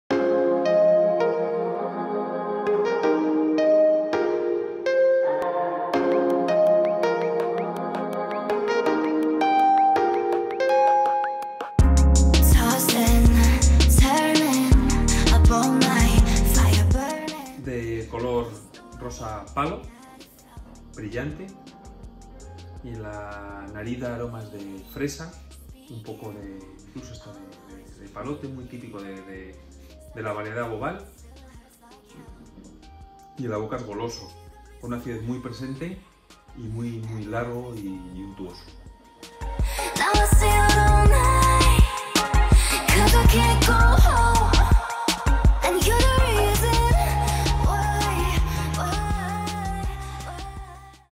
Finca del Mar ROSÉ
Pepe Hidalgo, Director Técnico de Bodegas Vicente Gandía nos presenta la video-cata de Finca del Mar Rosé.